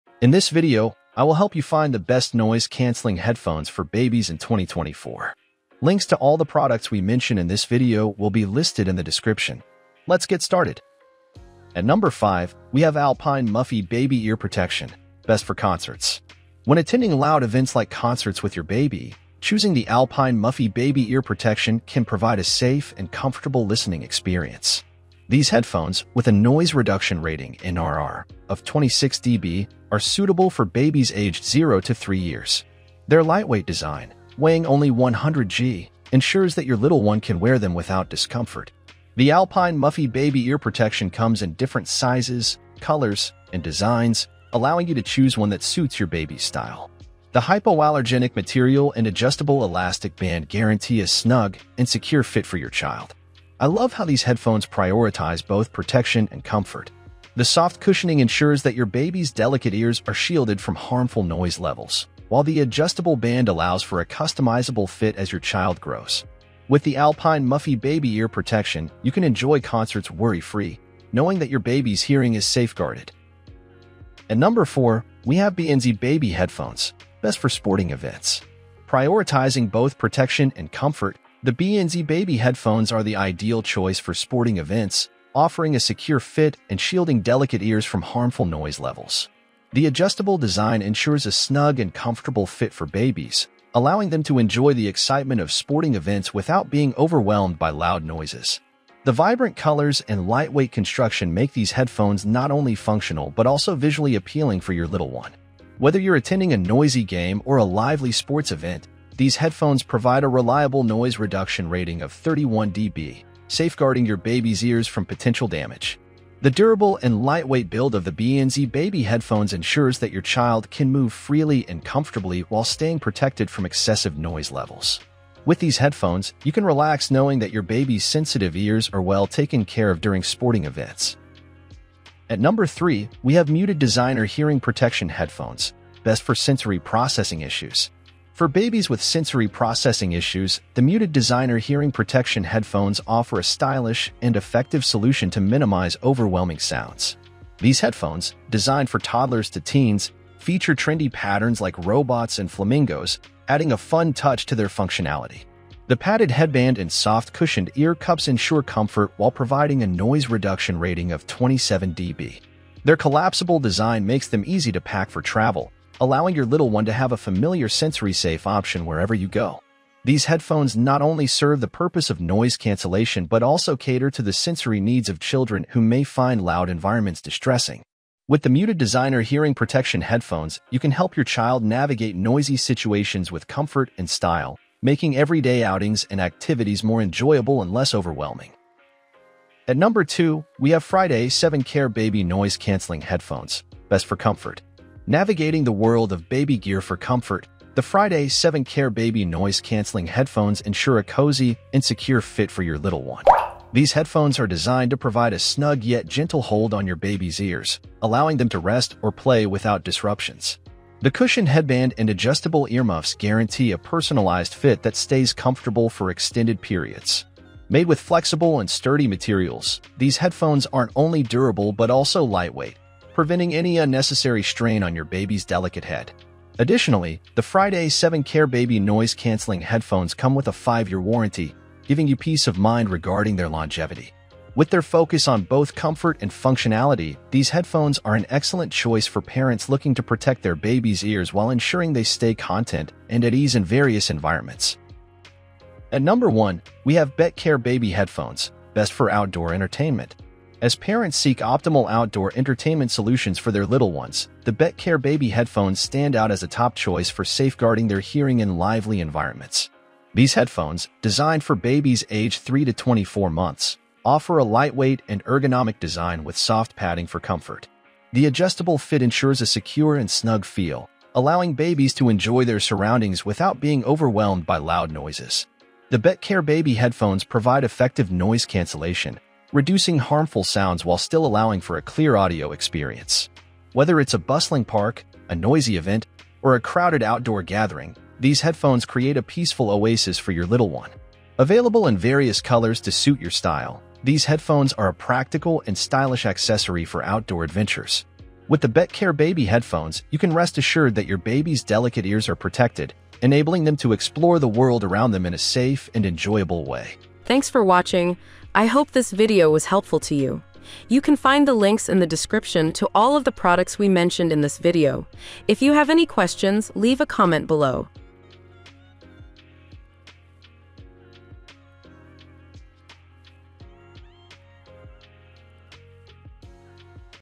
Best Noise Cancelling Headphones For Babies in 2024: Protecting Little Ears with Style and Comfort
► Links to the best noise cancelling headphones for babies we listed in this video:

► Alpine Muffy Baby Ear Protection Best For Concerts
► BANZ Baby Headphones
► Muted Designer Hearing Protection Headphones
► Friday 7Care Baby NoiseCanceling Headphones
► BBTKCARE Baby Headphones Best For Outdoor Entertainment

Today, we reviewed the 5 best noise cancelling headphones for babies you can buy in 2024. We prepared this list based on various tests, user reviews, and real life experiences. We tried to prepare a list that is suitable for budget conscious buyers that are looking for affordability and reliability at the same time. At the same time, we included some premium picks for those looking to get a high-end experience.